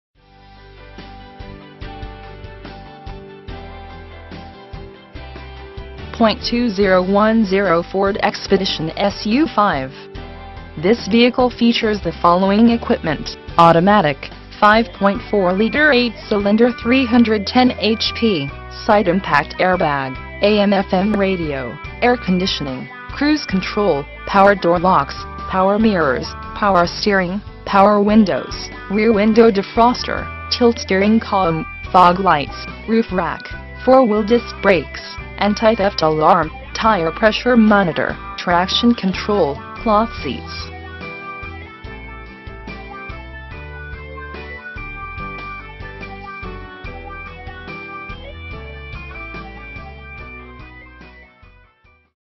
2010 Ford Expedition - Cals Park -n- Sell
900 E. Dowling Rd

For Sale: 2010 Ford Expedition
VIN: 1FMJK1G54AEA88248
Engine: 5.4-Liter 8-Cylinder 310HP
Drivetrain:
Transmission: automatic
Mileage: 32,114
Color: SILVER (exterior)  (interior)

Features:
2010 Ford Expedition S U V. This vehicle features the following equipment: automatic, 5.4-Liter 8-Cylinder 310HP, Side Impact Airbag, A M/FM Radio, Air Conditioning, Cruise Control, Power Door Locks, Power Mirrors, Power Steering, Power Windows, Rear Window Defroster, Tilt Steering Column, Fog Lights, Roof Rack, 4-Wheel Disc Brakes, Anti-Theft Alarm, Tire Pressure Monitor, Traction Control, Cloth Seats